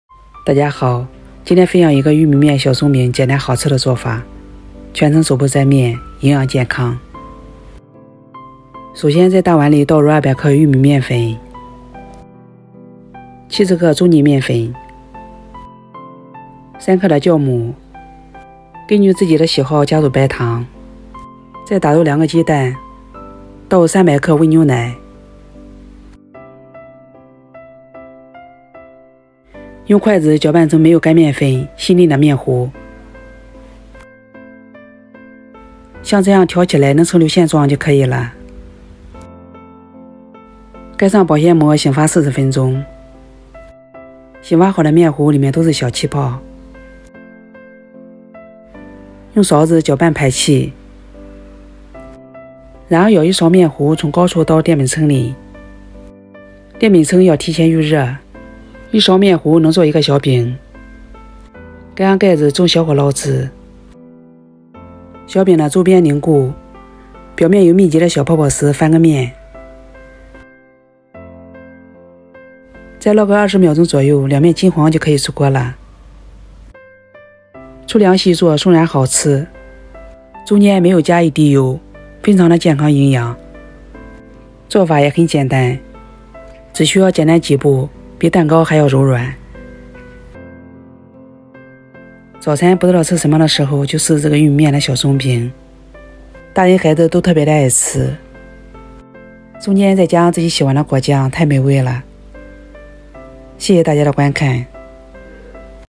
早餐玉米面小松餅全程手不沾面，只需要簡單兩步松軟好吃營養健康
No.3 | 萬能手抓餅生坯別買了，多做些冷凍保存能吃2個月，好吃省時方便

食譜,面食,早餐食譜,美食,玉米餅,菜譜,家常菜,中餐